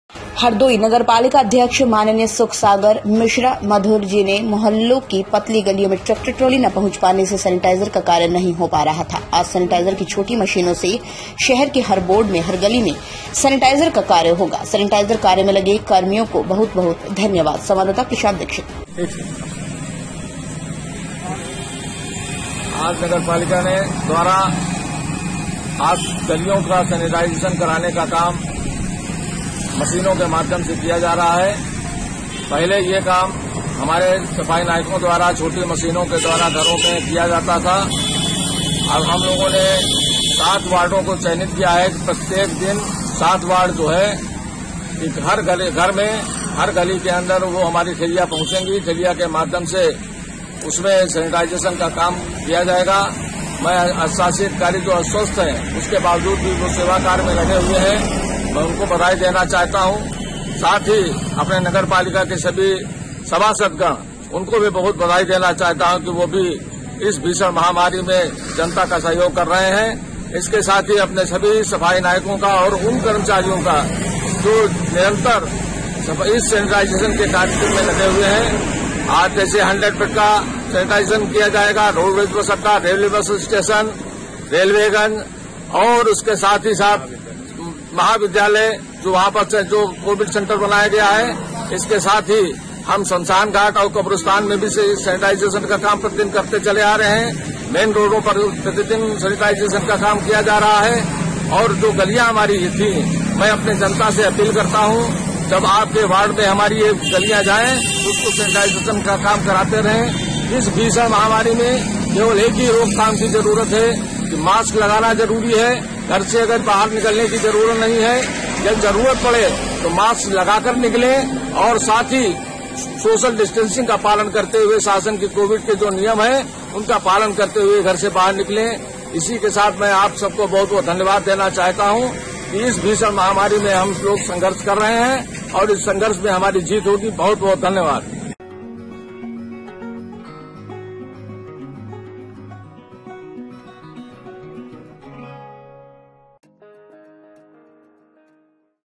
। Nonstop News । सेनेटाइजर की छोटी मशीनों से शहर के हर बार्ड में हर गली में सेनेटाइजर का कार्य ।
हरदोई
नगर पालिका अध्यक्ष मा.सुखसागर मिश्रा मधुर जी ने मोहल्लों की पतली गलियों में ट्रैक्टर ट्राली ना पहुँच पाने से सेनेटाइजर का कार्य नही हो पा रहा था आज सेनेटाइजर की छोटी मशीनों से शहर के हर बार्ड में हर गली में सेनेटाइजर का कार्य होगा सेनेटाइजर कार्य मे लगे कर्मियों को बहुत बहुत धन्यबाद ।

ईओ रविशंकर शुक्ला

संवाददाता प्रशान्त दिक्षित